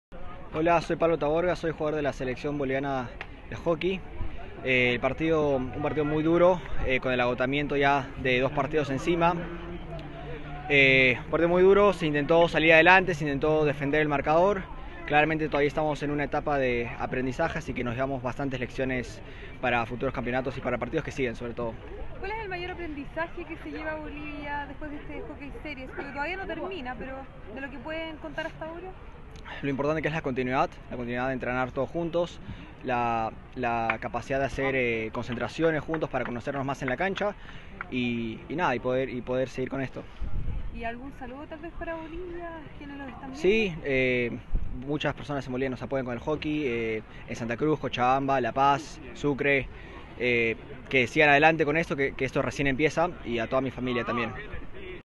Pablo TABORGA (BOL)
2018 Hockey Series Open – Santiago, Chile – 18 September 2018 - Chile 27 - Bolivia 0 - Pablo Taborga (CHI)